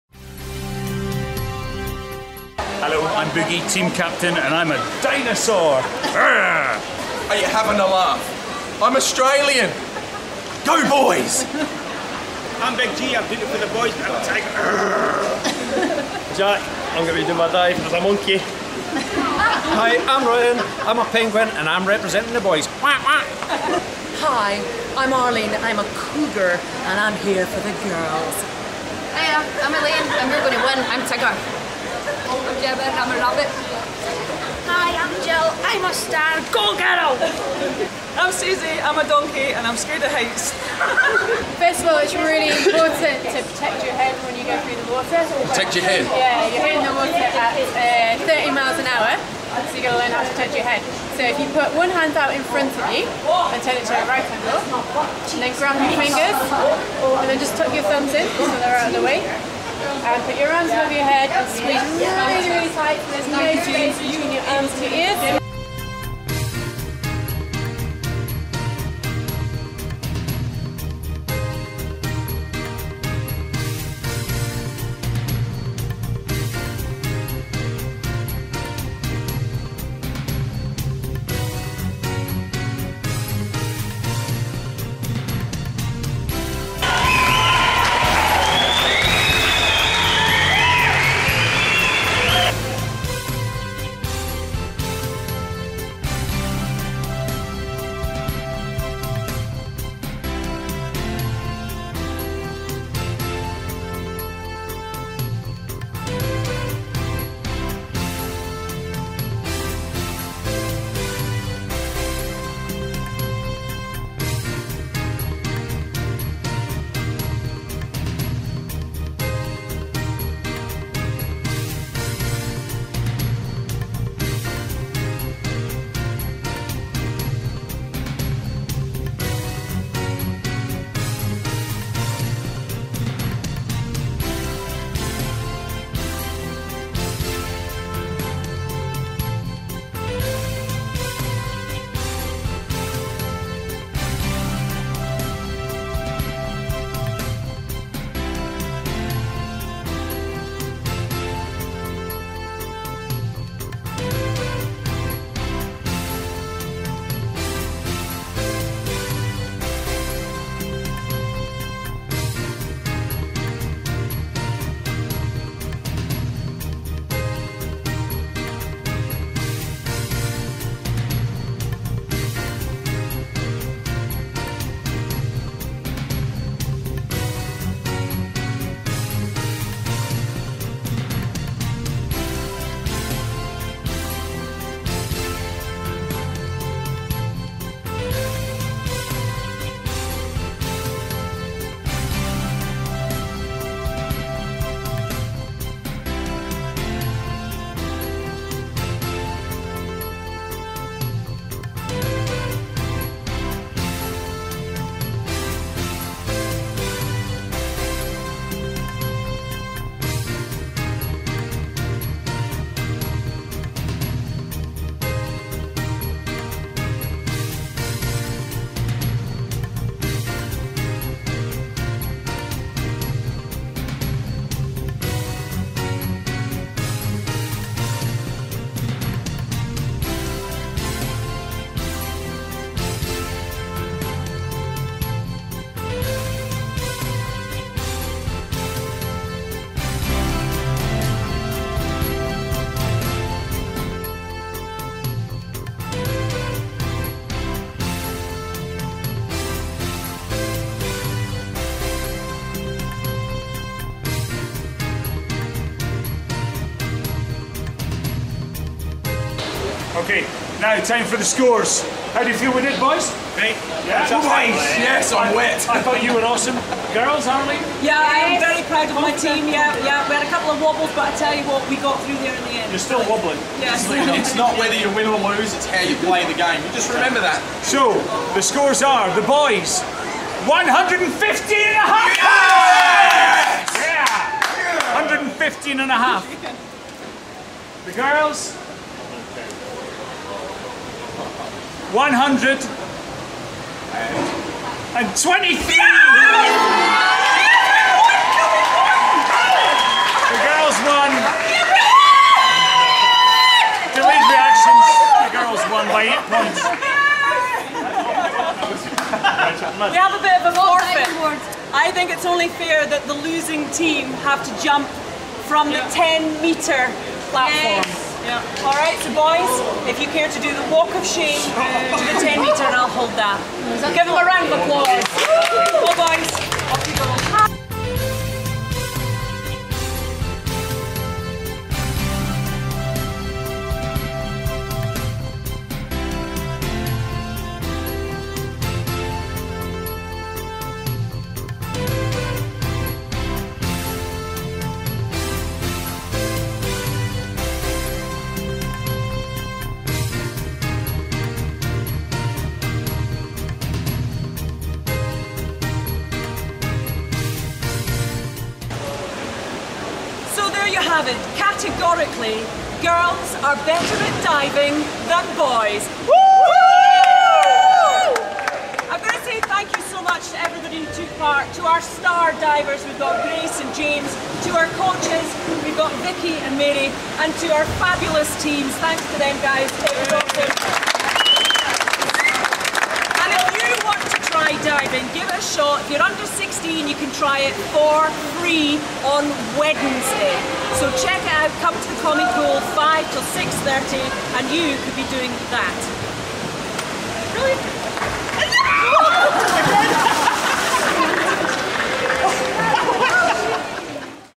The Onesie Diving Challenge
Boogie,Dingo and Arlene compete with Teams in Boys VS Girls Onesie Diving Challenge at Edinburgh Commonwealth Pool